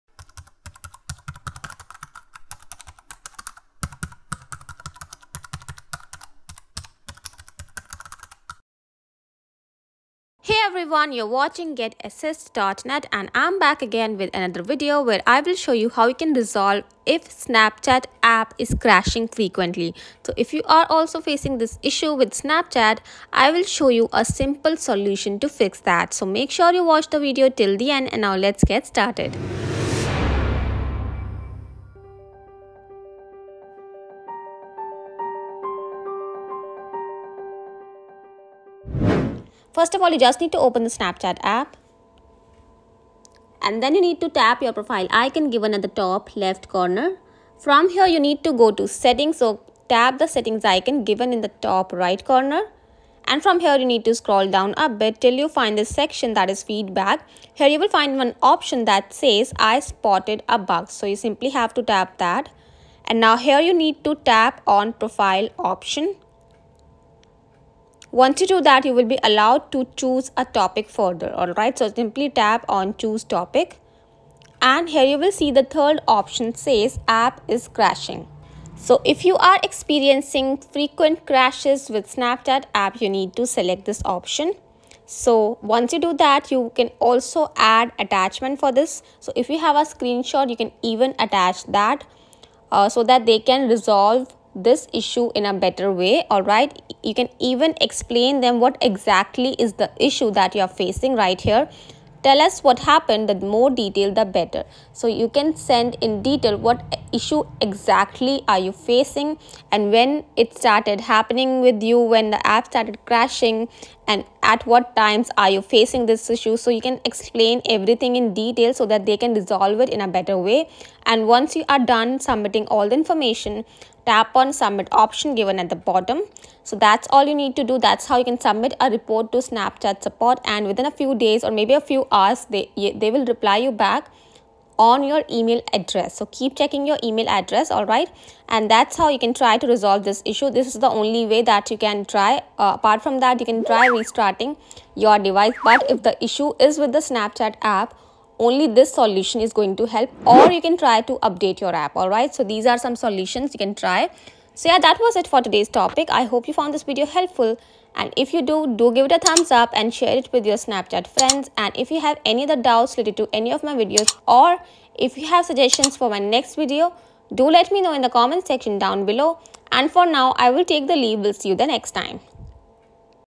Easy Ways to Fix Snapchat Keeps Crashing! [Proven Method]
if your Snapchat app is crashing then you need to follow the steps to fix this challenge.

0:40 Open the Snapchat app and select the profile icon in the upper left corner.
0:49 go to settings (gear icon).
0:57 Select the Feedback Option
1:06 Click on "Profile" and choose a topic.
1:20 The application has crashed.

📌Read Blog Here ⬇️⬇️
Why Does Snapchat Keep Crashing? How to Fix (5 Solutions)